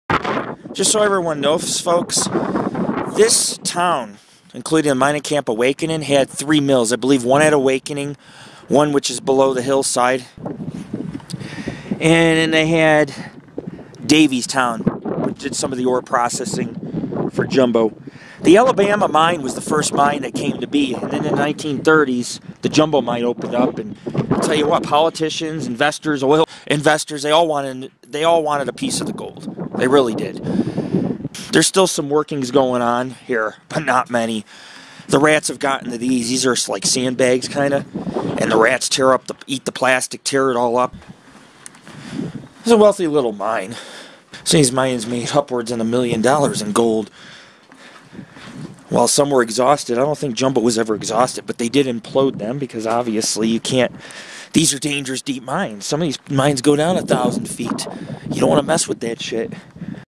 Jumbo Nevada - Part 2 "President Hoovers Forgotten City"
Mining begin here around 1910 but truly Jumbo did not grow into the town it became till around 1935 when the Jumbo Mine came to be. President Herbert Hoover visited this camp around 1937 and referred to it as a very impressing mining town. Mining actually continued up until the 1980's over the years time and time again a man by the name of George Austin leased the mine to multiple investors. Most investors were tired of the high cost, litigation and broke the lease. With that being said you had booms in 1941, 1947 and 1951. Jumbo also had a 30 ton amalgamation mill, dance hall, saloon, quite a few claims, glory hole and multiple residences. Today not much remains of Jumbo but the views are stellar and their are plenty of remnants of the past to see including the cabins where the miners lived and even the admin building where they stored their boots and hard hats. The set of snow-capped peaks is the Santa Rosa Range sorry I said Santa Clara a few times by accident this place excited me. While not much remains ill take our viewers throughout the mine, mill and cabin sites of this haunted gold mining town which in its hey day was quite the marvel before it went bust.